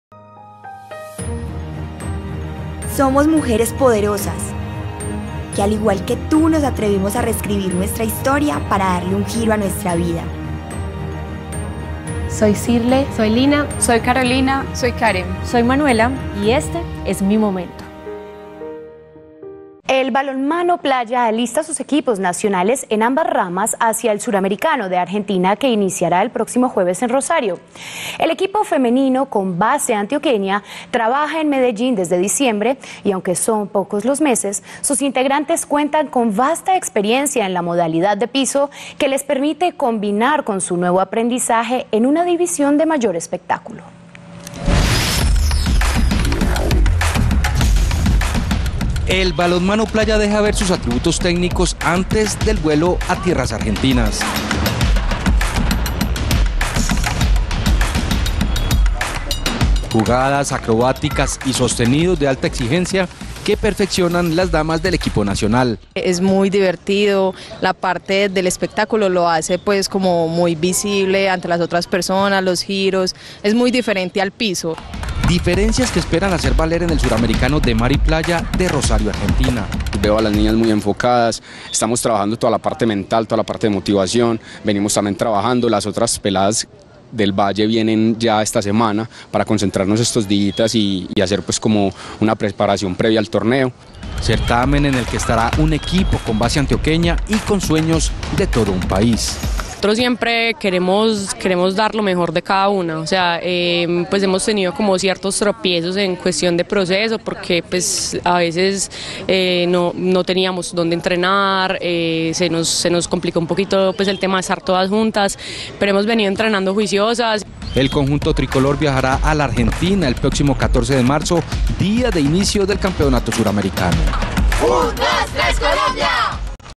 Balonmano playa alista sus equipos nacionales hacia el Suramericano
El balonmano playa alista sus equipos nacionales en ambas ramas hacia el Suramericano de Argentina que iniciará el próximo jueves en Rosario.
El equipo femenino con base antioqueña trabaja en Medellín desde diciembre y aunque son pocos meses, sus integrantes cuentan con vasta experiencia en la modalidad de piso que les permite combinar con su nuevo aprendizaje en una división de mayor espectáculo.